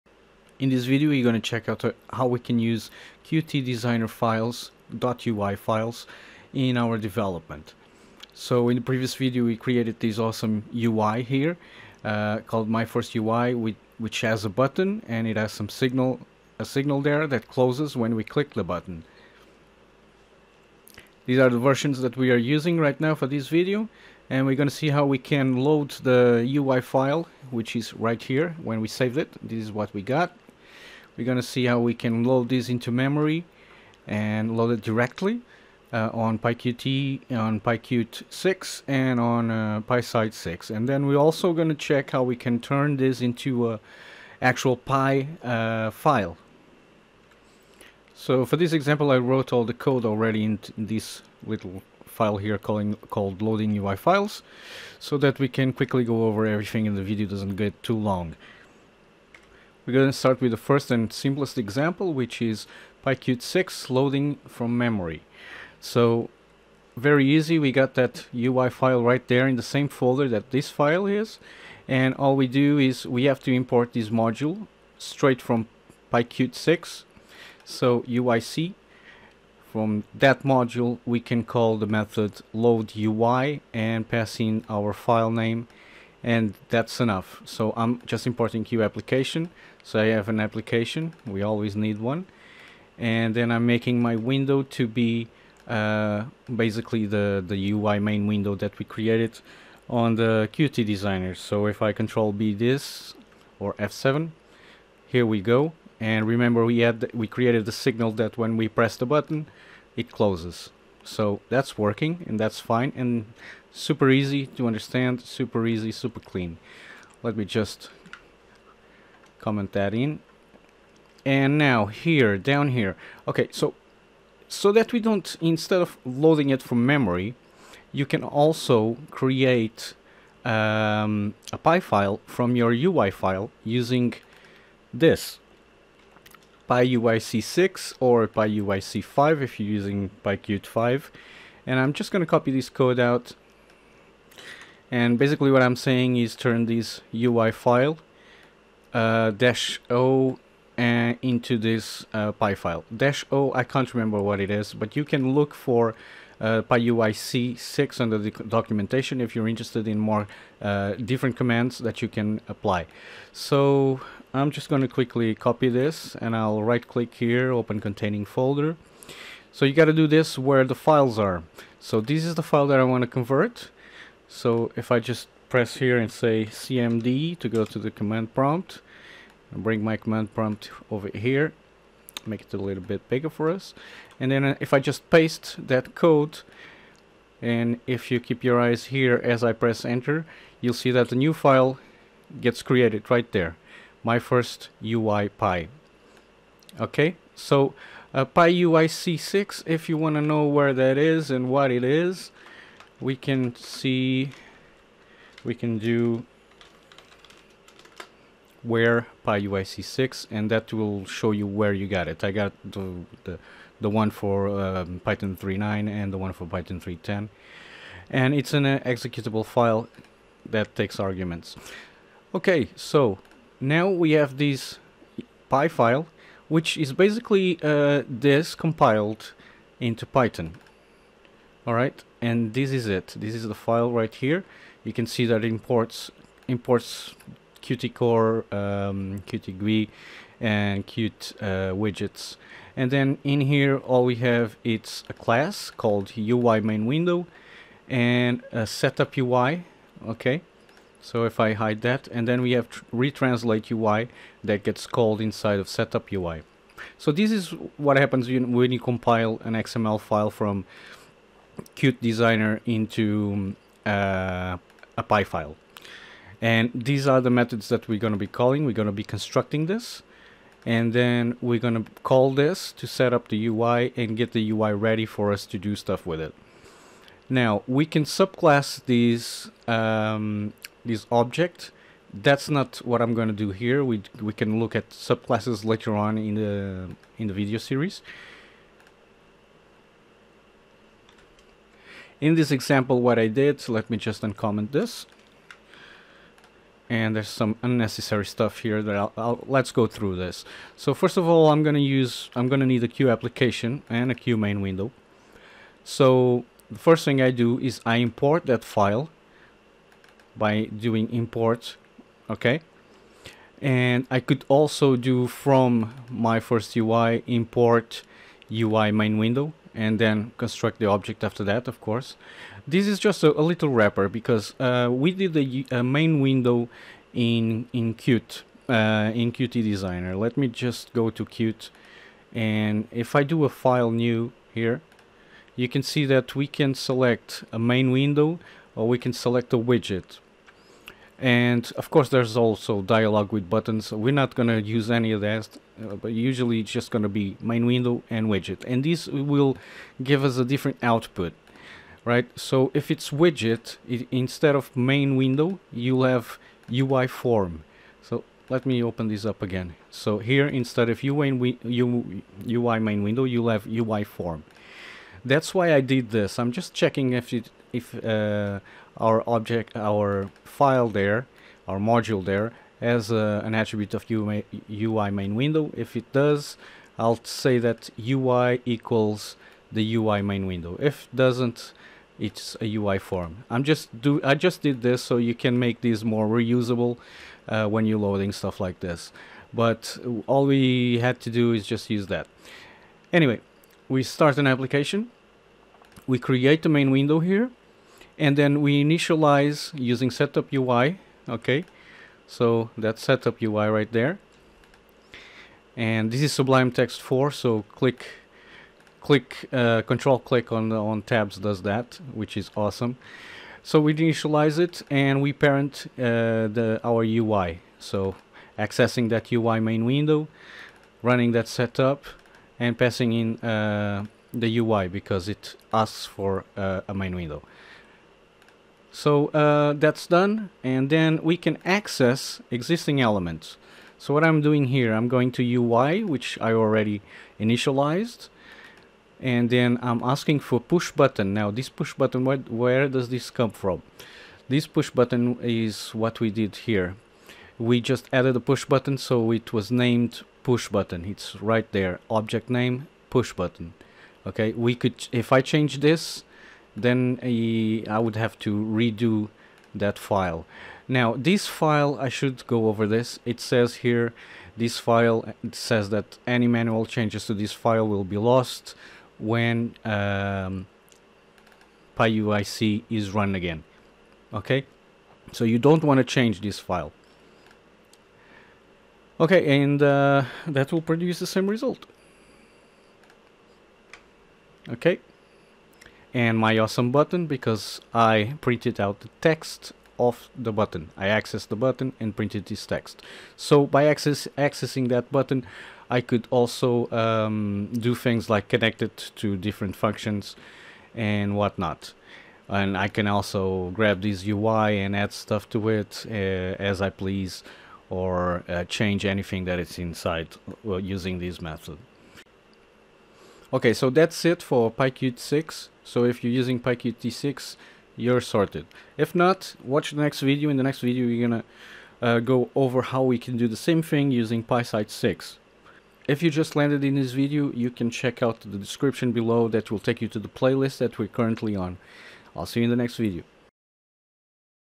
PyQt6 QtDesigner .ui files
In this video we will learn how to use PyQt6 QtDesigner .ui files.